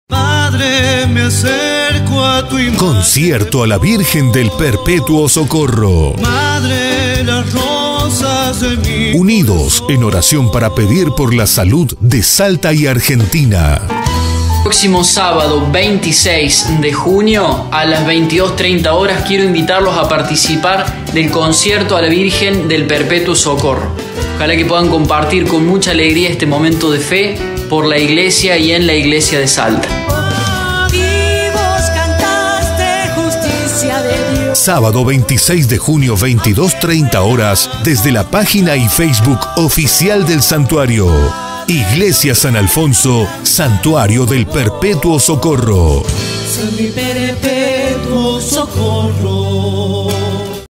Concierto a la Virgen del Perpetuo Socorro
¡Unidos en oración para pedir por la salud de Salta y Argentina!
►Sábado 26 de junio, 22.30 hs. (modalidad virtual)
Será transmitido por nuestra página de Facebook y nuestro sitio web